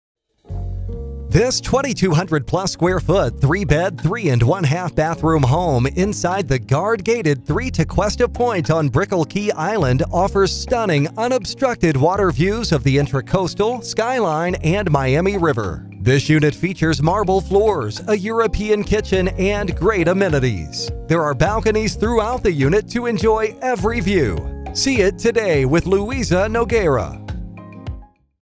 848 Brickell Key Drive Tequesta Three Unit 706 Miami, FL | ColdwellBankerHomes.com
848 Brickell Key Drive Tequesta Three Unit 706, Miami, FL

This three bedroom, three and one half bathroom home offers stunning unobstructed water views of the intracoastal skyline, Miami River and much more. This unit features marble floors, a European kitchen, crown molding, professional custom designed furniture for TV and Bar at Three Tequesta Point ins